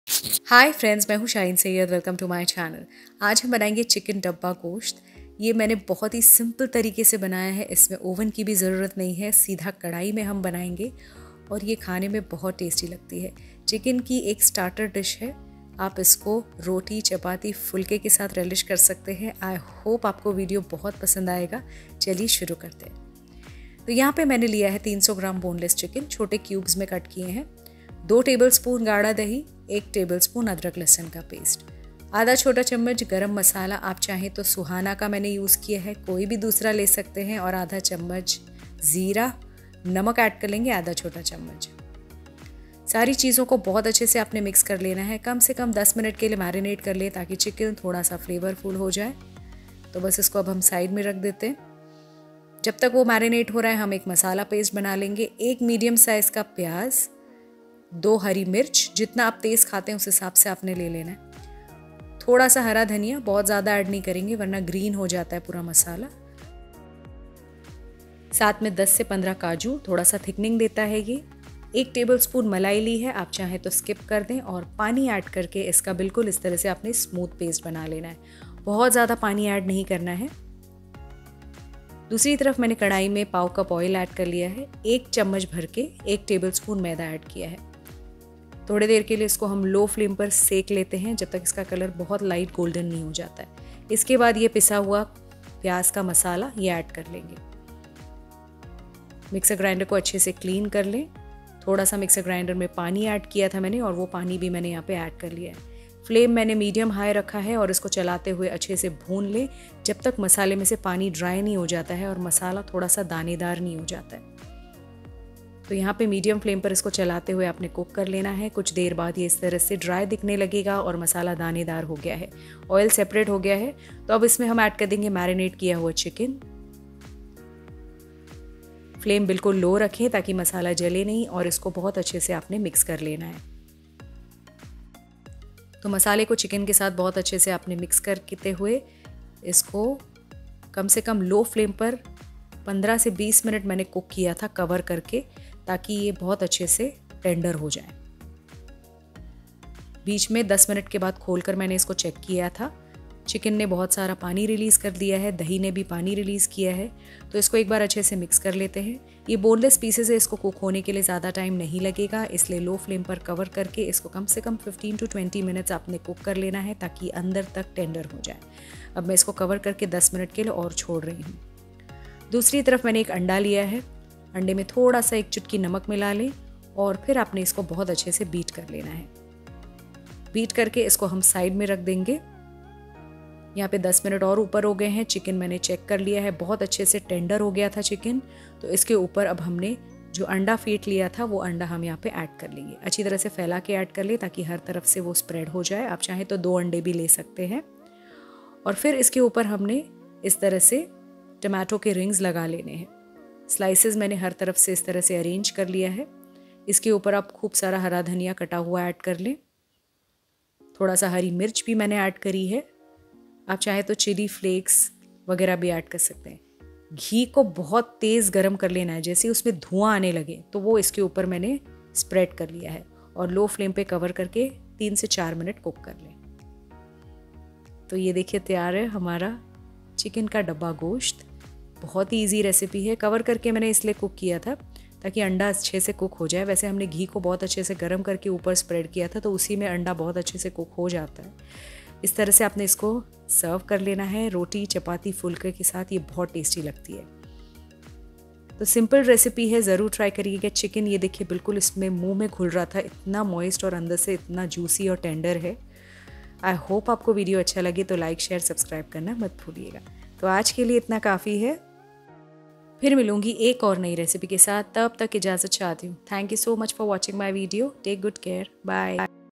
Dabba Gosh With Chicken Recipe | Easy chicken ka डब्बा गोश्त की रेसिपी famous dabba gosh recipe
Dabba Chicken Starter Recipe | Easy chicken ka डब्बा गोश्त की रेसिपी

ingredients
300 grams chicken
1 tbsp ginger garlic paste
2 tbsp yoghurt
1/2 tsp garam masala powder
1/2 tsp cumin seed powder
1/2 tsp salt
1 tbsp maida
1 medium onion
2 green chilli
fee coriander leaves
15 cashews
1 tbsp full cream
water to make paste
1 tomato sliced
1 egg
chopped coriander
slitted green chillies
2 tbsp desi pure ghee